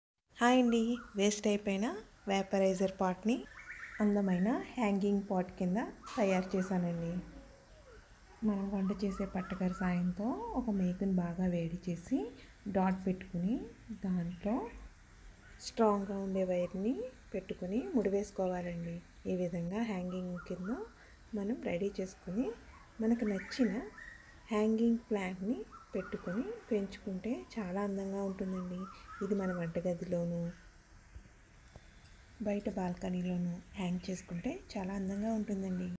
Beautiful Hanging Plastic Pot From Damaged Vaporizer Part || Hanging Plant|| How to make Hanging pot
vaporizer part
homemade Hanging pot Plant
trendy Hanging Plant